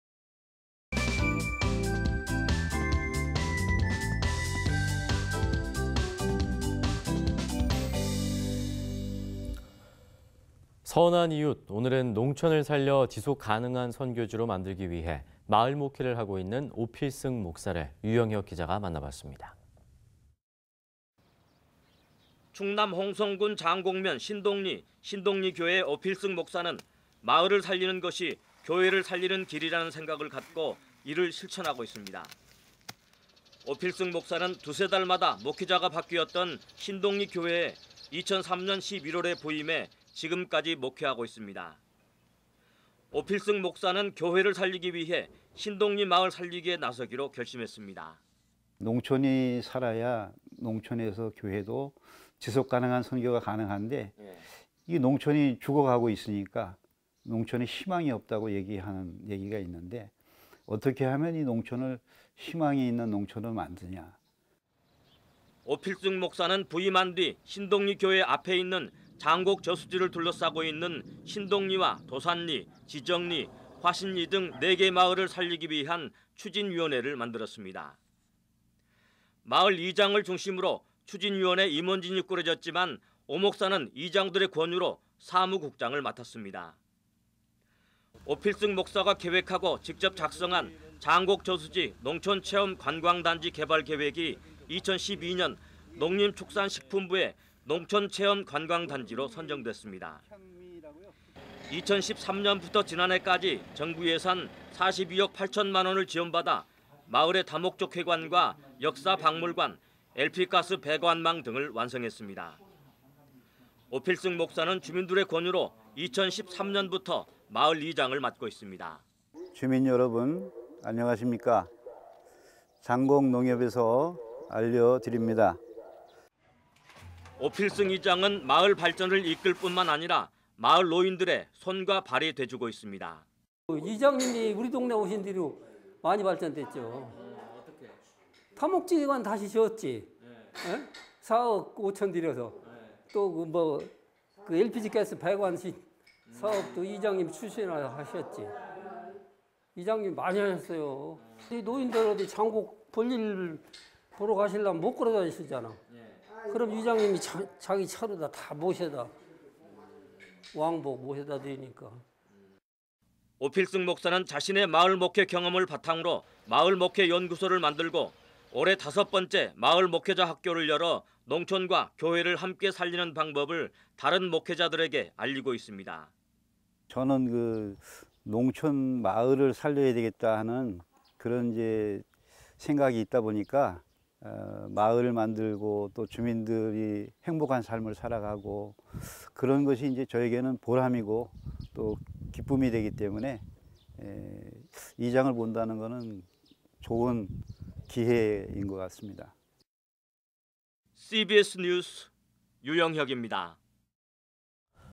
[CBS 뉴스] 선한이웃-농촌과 농촌교회 살리기 나선 오필승 목사
선한이웃, 오늘은 농촌을 살려 지속가능한 선교지로 만들기 위해
마을 목회를 하고 있는 오필승 목사를 유영혁 기자가 만나봤습니다.

충남 홍성군 장곡면 신동리 신동리교회 오필승 목사는
마을을 살리는 것이 교회를 살리는 길이라는 생각을 갖고
이를 실천하고 있습니다.

오필승 목사는 두세달마다 목회자가 바뀌었던 신동리교회에
2003년 11월에 부임해 지금까지 목회하고 있습니다.

오필승 목사는 교회를 살리기 위해 신동리마을 살리기에
나서기로 결심했습니다.

[인터뷰]오필승 목사/신동리교회

"농촌이 살아야 농촌에서 교회도 지속가능한 선교가
가능한데 농촌이 죽어가고 있으니까 농촌이 희망이 없다는
애기가 있는데 어떻게 하면 농촌을 희망이 있는 농촌으로
만드느냐

오필승 목사는 부임한 뒤 신동리교회 앞에 있는
장곡저수지를 둘러싸고 있는 신동리와 도산리, 지정리,
화신리 등 4개 마을을 살리기 위한 추진위원회를
만들었습니다.

마을 이장을 중심으로 추진위원회 임원진이 꾸려졌지만
오 목사는 이장들의 권유로 사무국장을 맡았습니다.

오필승 목사가 계획하고 직접 작성한 장곡저수지
농촌체험관광단지 개발계획은 2012년 농림축산식품부의
농촌체험관광단지로 선정됐습니다.

2013년부터 지난해까지 정부예산 42억8천만원을 지원받아
마을의 다목적회관과 역사박물관,LP가스 배관망 등을
완성했습니다.

오필승 목사는 주민들의 권유로 2013년부터는 마을 이장을
맡고 있습니다.

[현장음]
5209 "신동리 주민여러분 안녕하십니까? 장곡농협에서
알려드립니다.~Fade out

오필승 이장은 마을 발전을 이끌 뿐만아니라
마을 노인들의 손과 발이 돼주고 있습니다.

[인터뷰]이권형/신동리 노인회장
0625 선한이웃 4728
"이장님이 우리동네 오신뒤로 많이 발전됐죠 다목정회관
다시 지었지 4억5천들여서 LPG가스 배관망 사업 이장님이
추진하셨지 이장님이 많이 하셨어요 4744
5048 노인들 여기 장곡 볼일 보러 가시려면 못 걸어다니
잖아요 이장님이 자기 차로 다 왕복으로 모셔다 드리니까
5104

오필승 목사는 자신의 마을목회 경험을 바탕으로
마을목회연구소를 만들고 올해 5번째 마을목회자학교를 열어
농촌과 교회를 함께 살리는 방법을 다른 목회자들에게
알리고 있습니다.

교회 목회자로 마을 이장으로 교회와 마을을 위해
봉사하고 있지만 침체에 빠진 농촌과 농촌교회를 살리는
것이기에 지치지 않는다고 얘기합니다.

5428 "저는 농촌마을을 살려야 되겠다 하는 그런 생각이
있다 보니까 마을을 만들고 주민들이 행복한 삶을 살아가고
그런 것이 보람이고 기쁨이되기 때문에 이장을 본다는 것은
좋은 기회인 것 같습니다.